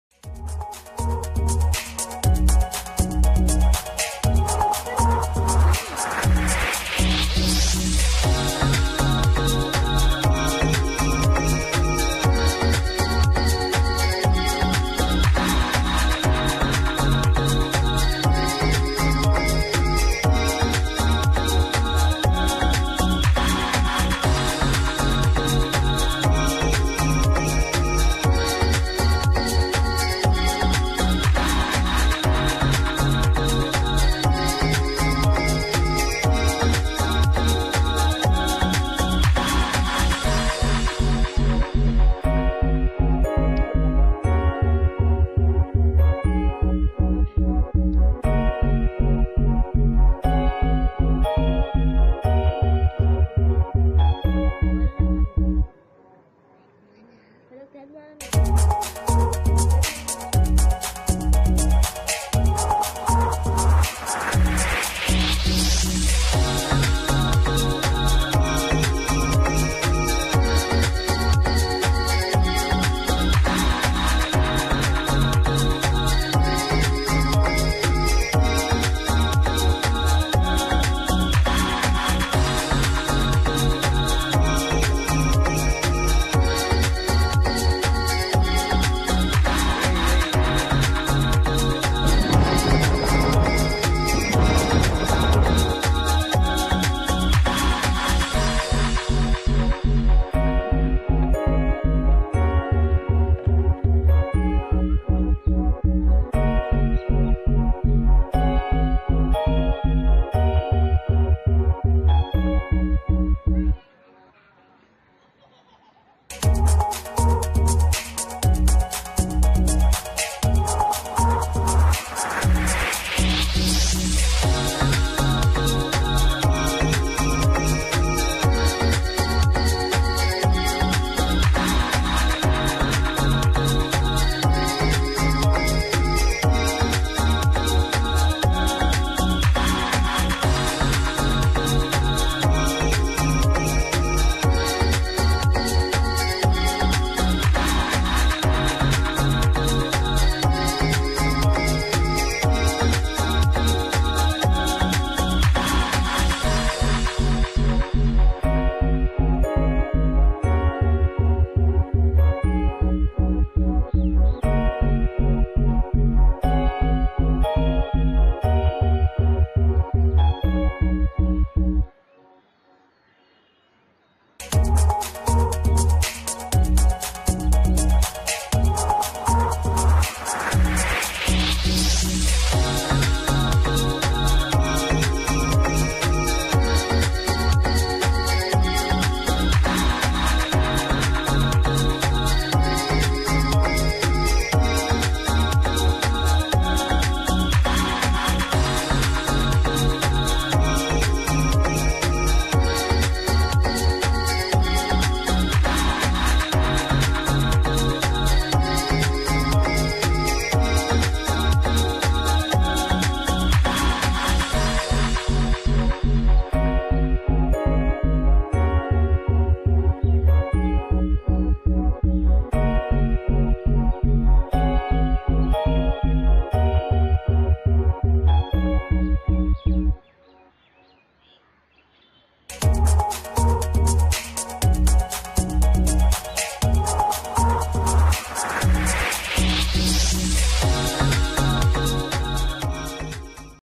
leak turtle .
Turtle view, food for plant, fish and turtles